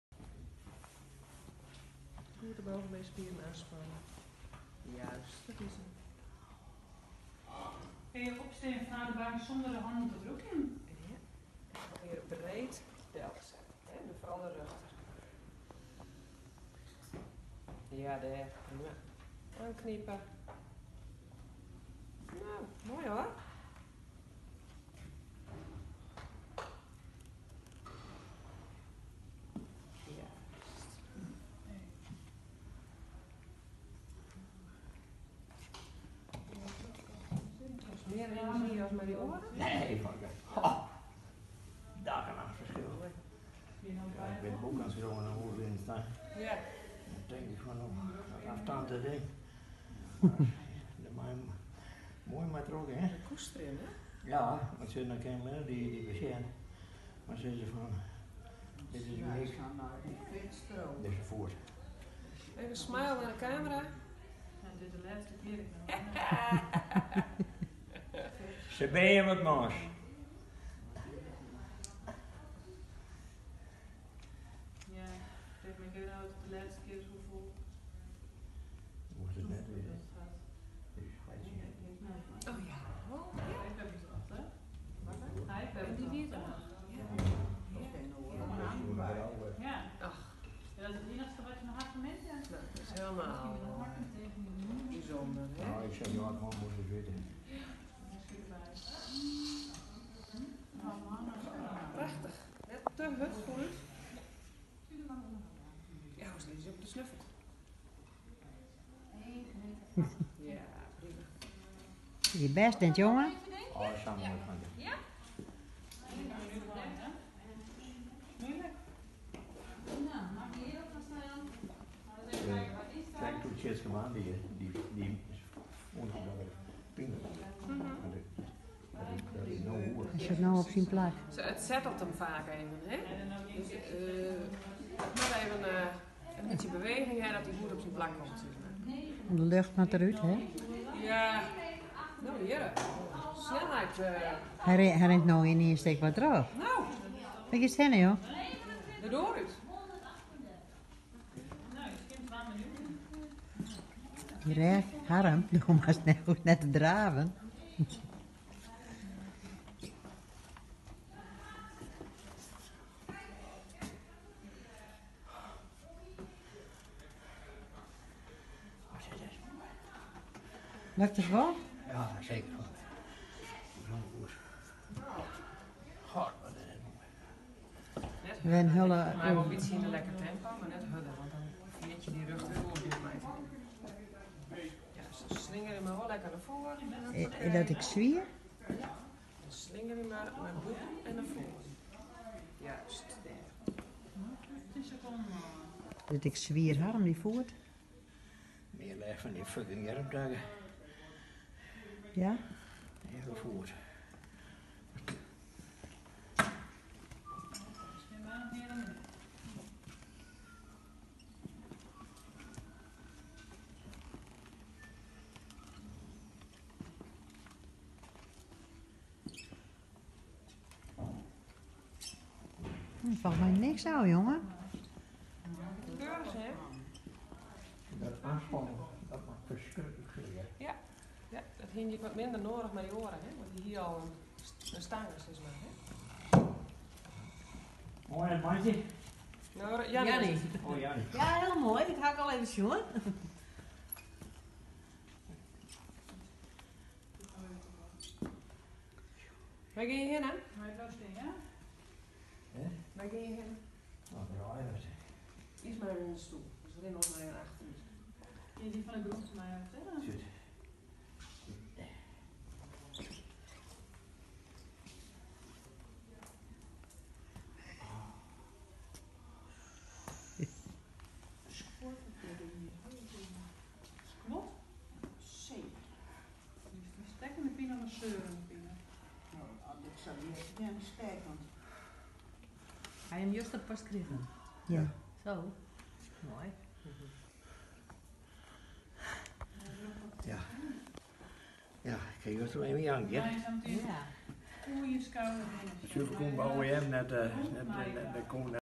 Na bijna 4 jaar weer lopen
Na bijna 4 jaar loop ik weer. 16 juli 2019 lyndenstein 💪💪🖐🖐